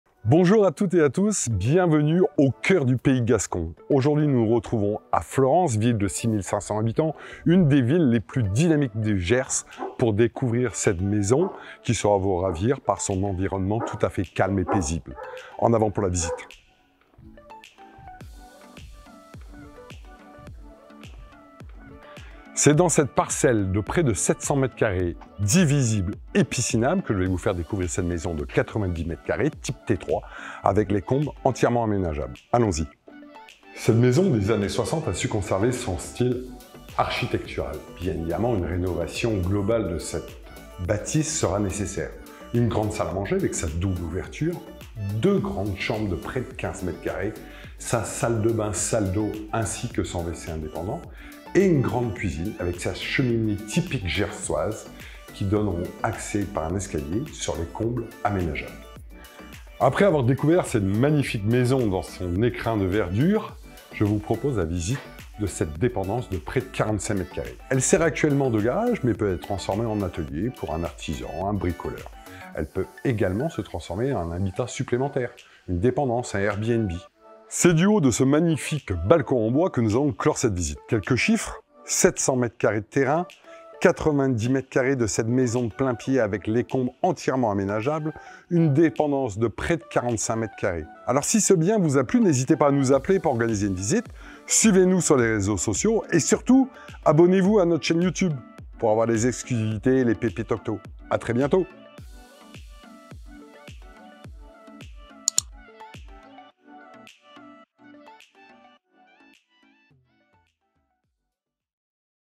MAISON FLEURANCE
Dans le quartier résidentiel de FLEURANCE, Venez découvrir cette maison des années 1970 située dans une rue calme et verdoyante.
Cette maison de 90m² au sol vous permettra de vous projeter dans une rénovation à votre image.
l étage est totalement aménageable.
Une dépendance de 65 m² pouvant servir de garage, annexe voir même de gite sur une surface de terrain de 750m² vous permettra de de profiter du paysage.
Le bien est divisible avec un double accès sur la parcelle.